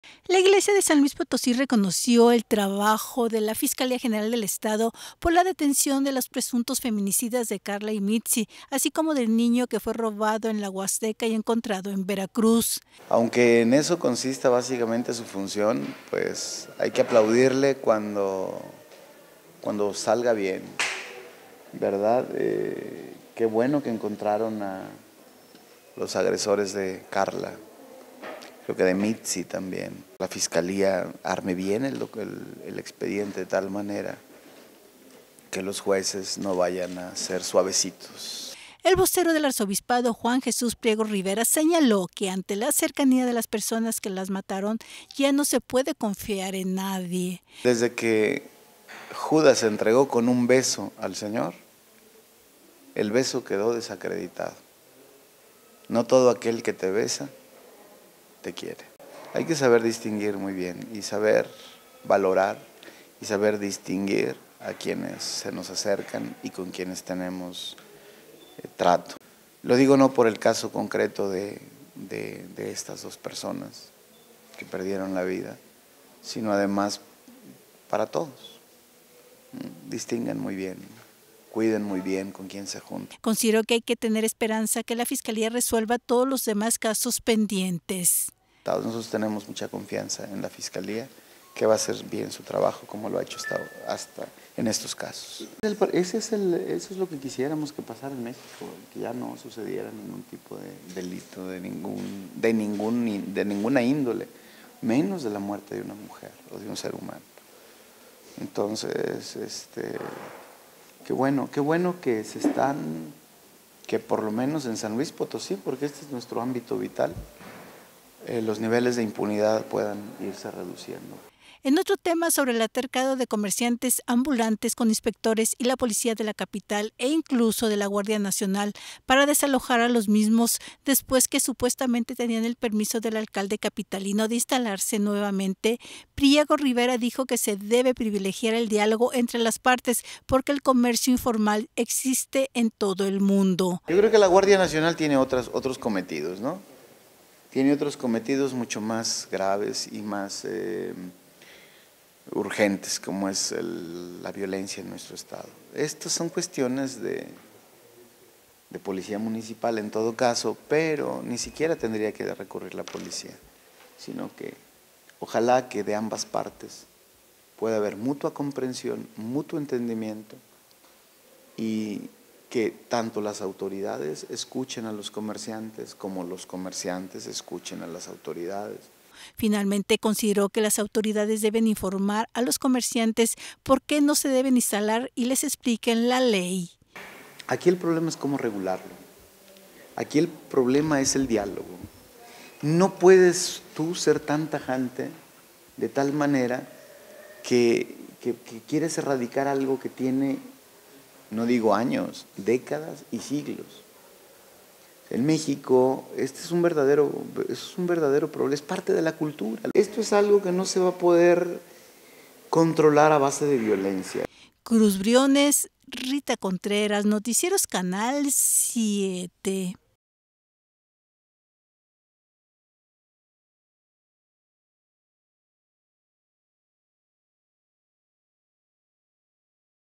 La Iglesia Católica aplaude acciones de la Fiscalía del Estado de SLP.
SLP.- La Iglesia de San Luis Potosí reconoció el trabajo de la Fiscalía General del Estado por la detención de los presuntos feminicidas de "Karla" "Mitzy" y el niño que fue encontrado en Veracruz.

El vocero del Arzobispado, Juan Jesús Priego Rivera señaló que ante la cercanía de las personas que las mataron ya no se puede confiar en nadie.

Consideró que hay que tener esperanza que la Fiscalía resuelva los demás casos pendientes.

En otro tema, sobre el altercado de comerciantes ambulantes, con inspectores y la policía de la capital e incluso de la guardia Nacional para desalojar a los mismos después que supuestamente tenían el permiso del alcalde capitalino de instalarse nuevamente, Priego Rivera dijo que se debe privilegiar el diálogo entre la partes, porque el comercio informal existe en todo el mundo.

Finalmente consideró que las autoridades deben informar a los comerciantes porque no se deben instalar y les expliquen la Ley.